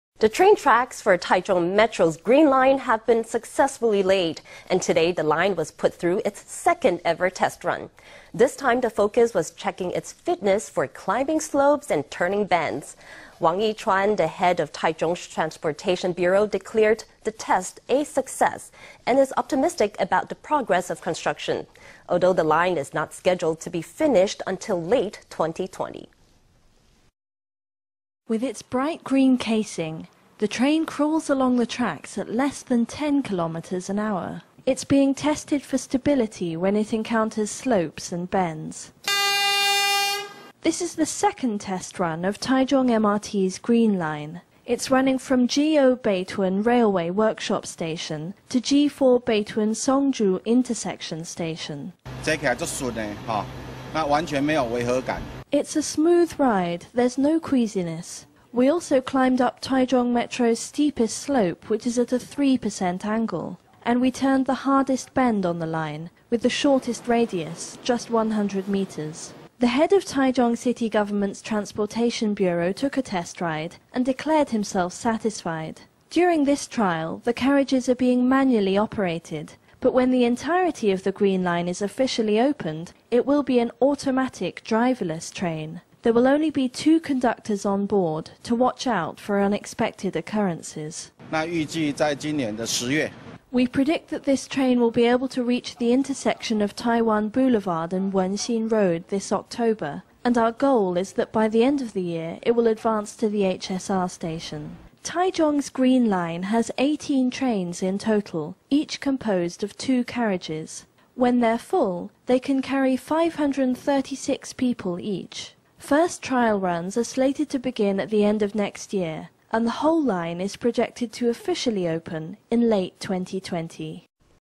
Taichung Metro green line passes second test with flying colors
The train tracks for Taichung Metro''s green line have been successfully laid, and today the line was put through its second ever test run. This time the focus was on checking its fitness for climbing slopes and turning bends. Wang I-chuan, the head of Taichung''s Transportation Bureau, declared the test a success, and is optimistic about the progress of construction, although the line is not scheduled to be finished until late 2020. With its bright green casing, the train crawls along the tracks at less than 10 kilometers an hour. It’s being tested for stability when it encounters slopes and bends.This is the second test run of Taichung MRT’s green line. It’s running from G0 Beitun Railway Workshop station to G4 Beitun Songzhu intersection station.Wang I-chuanTransportation Bureau Head, Taichung City Gov.It’s a smooth ride. There’s no queasiness. We also climbed up Taichung metro’s steepest slope, which is at a 3 percent angle. And we turned the hardest bend on the line, with the shortest radius, just 100 meters.The head of Taichung City Government’s Transportation Bureau took a test ride and declared himself satisfied. During this trial the carriages are being manually operated, but when the entirety of the green line is officially opened, it will be an automatic driverless train. There will only be two conductors onboard to watch out for unexpected occurrences.Wang I-chuanTransportation Bureau Head, Taichung City Gov.We predict that this train will be able to reach the intersection of Taiwan Boulevard and Wenxin Road this October, and our goal is that by the end of the year it will advance to the HSR station.Taichung’s green line has 18 trains in total, each composed of two carriages. When they’re full, they can carry 536 people each. First trial runs are slated to begin at the end of next year, and the whole line is projected to officially open in late 2020.